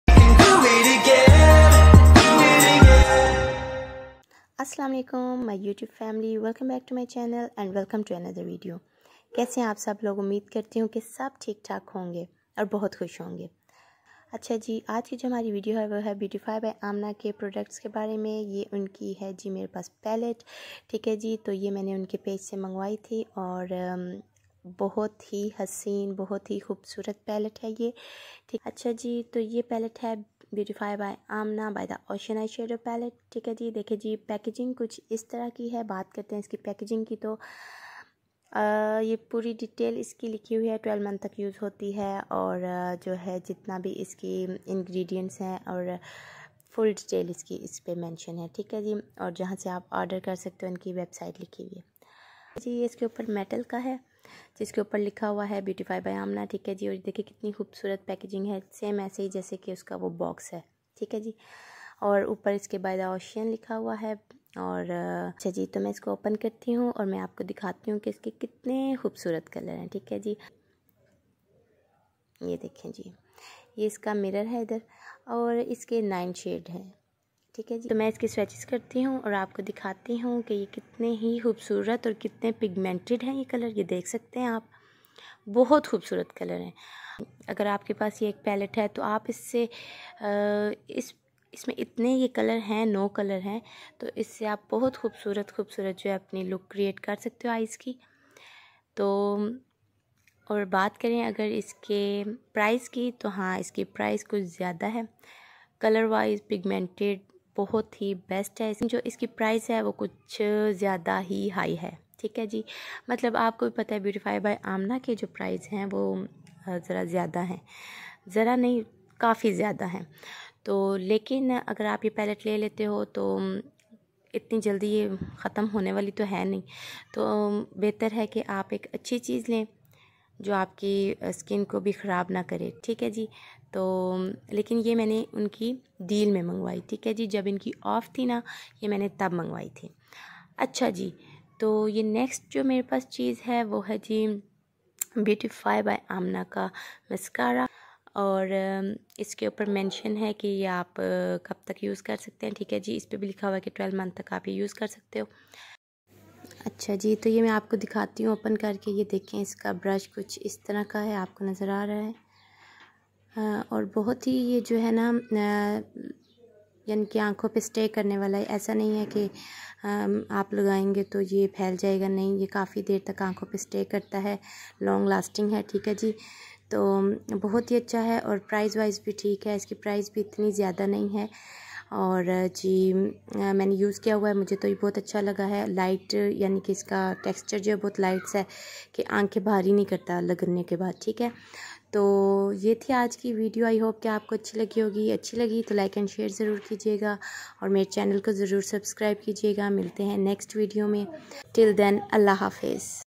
Beautify by amna Eyeshadow palette || Honest Review
Hello beautiful girls..kasy ho sab...i hope k sab thek hon gy. ❤️

Aj ki is video m mein ap k sath review share kron gi eyeshadow palette ka jo k bba by suleman by the ocean eyeshadow palette hai. i hope k ap ko video achi lgy gi. so keep watching and keep supporting me 😊